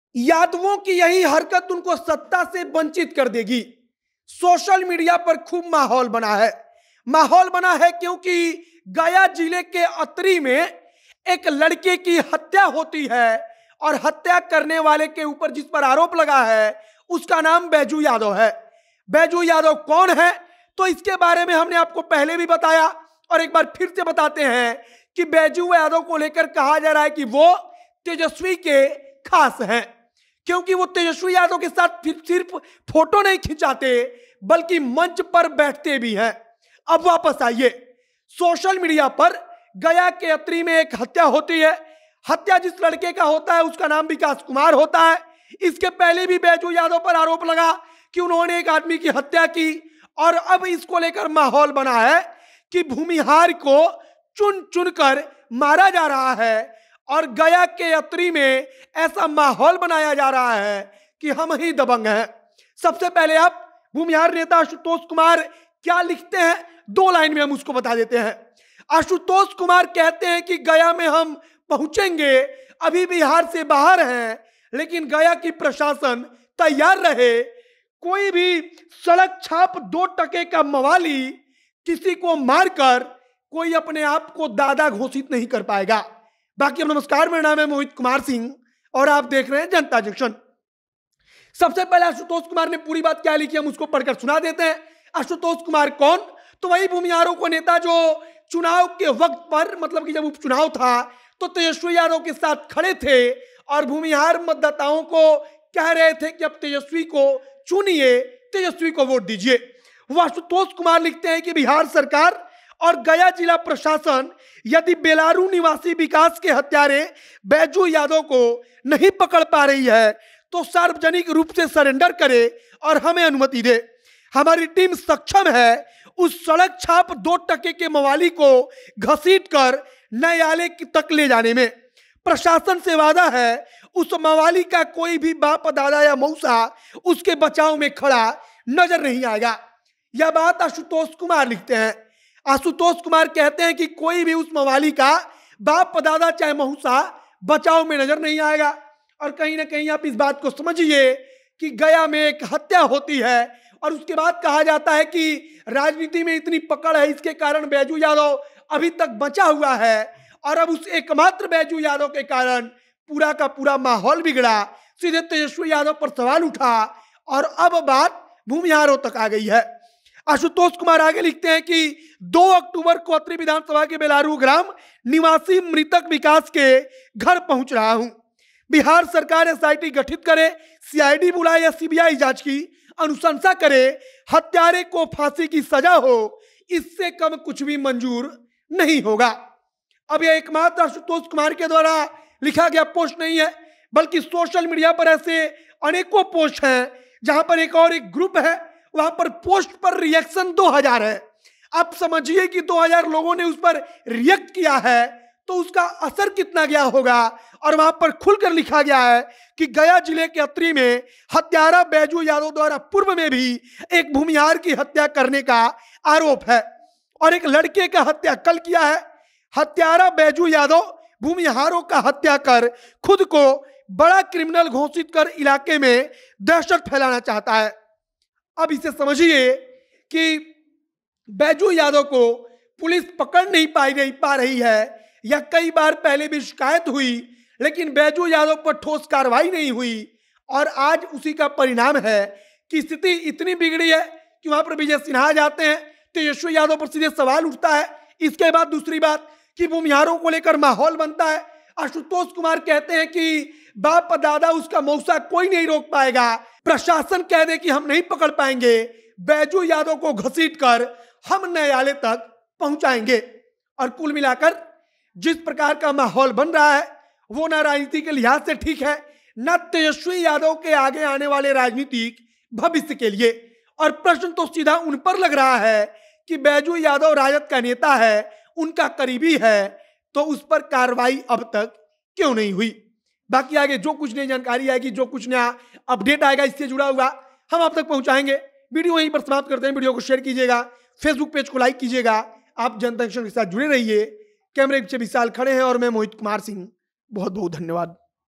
Tejashwi का नाम बदनाम कर रहे Yadav, अब तो Bhumihar नेताओं ने माहौल बना दिया! Baiju Yadav
यादव ही तेजस्वी को हरवाएंगे, भूमिहार नेता आशुतोष कुमार तो बैजू यादव के बाप, दादा, मौसा सब एक कर रहे हैं! एक बैजू यादव के कारण भूमिहार और यादव का माहौल बना!